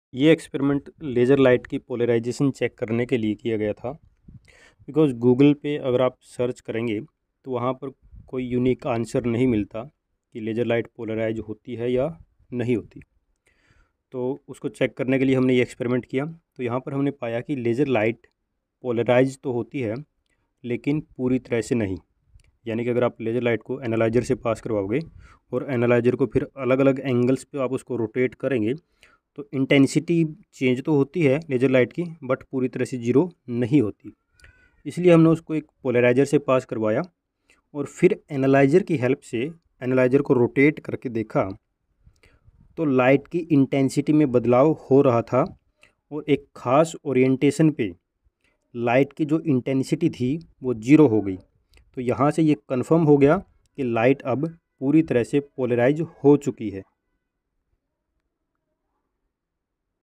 Experiment Laser light polarized or not ??class12
an experiment was performed to check polarization of Laser light